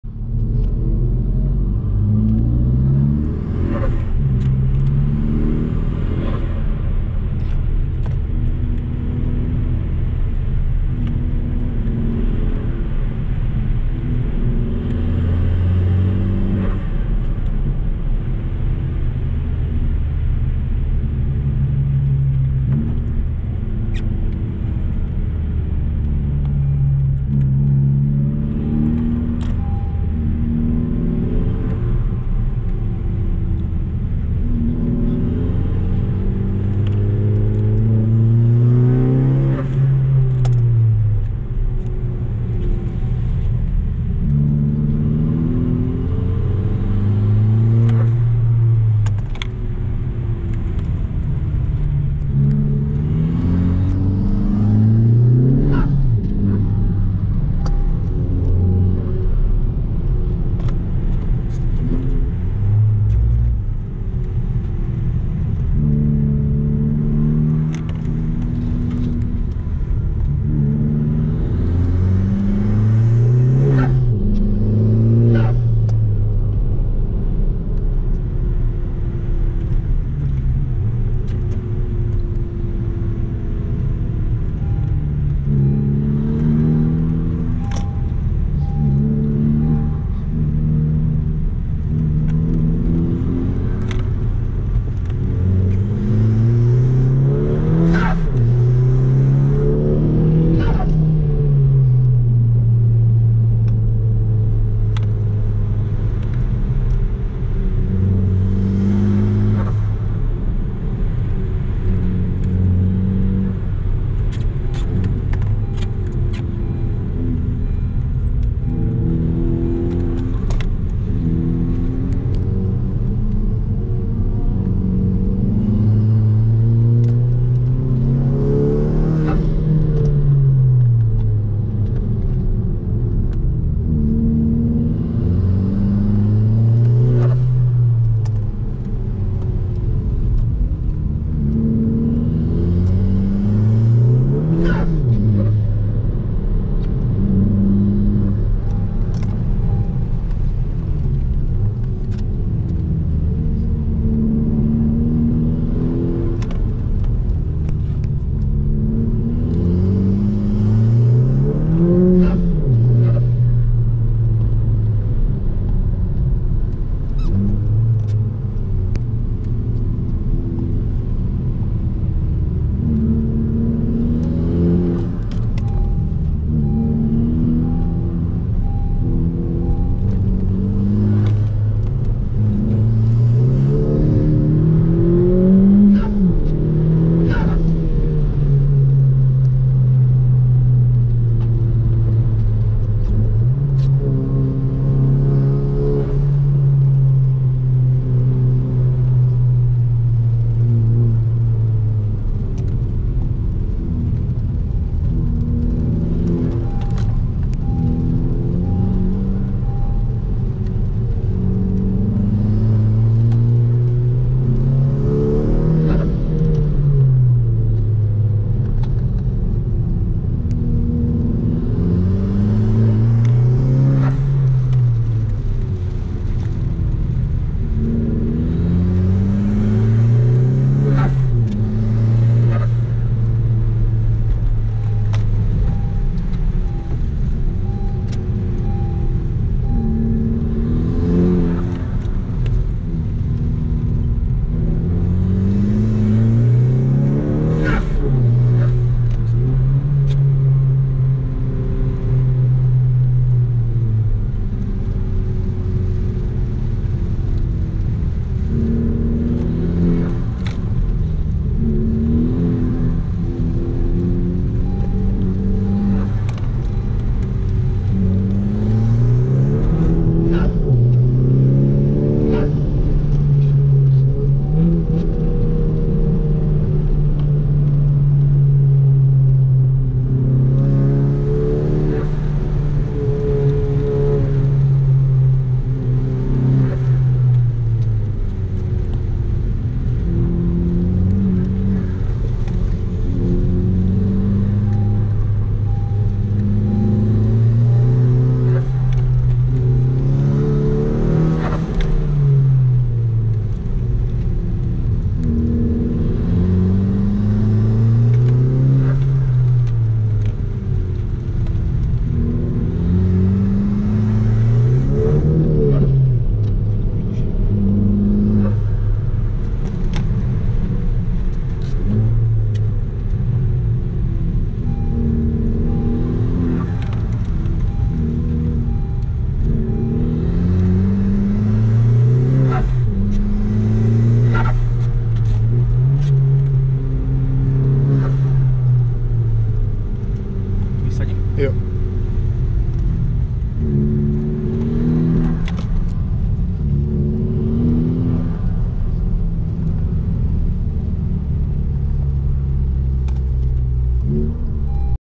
Honda Civic 9G Type-R on wet SteelRing (Onboard)
I recorded my friend in action during Track&Safety event on Silesia Steel Ring in Třinec - Czech Republic. Custom modified Civic Type-R is really funny toy for this roads, but circuit is primary for go karts and there are lot of short sections.

Nedávno na Steel ringu v Třinci proběhla akce Track&Safety, která sice byla celá za doprovodu vydatného deště, ale i tak si to všichni maximálně užili. Na jednu z jízd sem sednul do auta k Honzovi a natočil ho jak si užívá jízdu se svou Hondou Civic Type-R 9.generace, která má za sebou již několik úprav směrem k performance stylu. Bohužel byla trať kompletně prolitá vodou, takže žádné ohně nečekejte, ale jinak to bylo parádní svezníčko.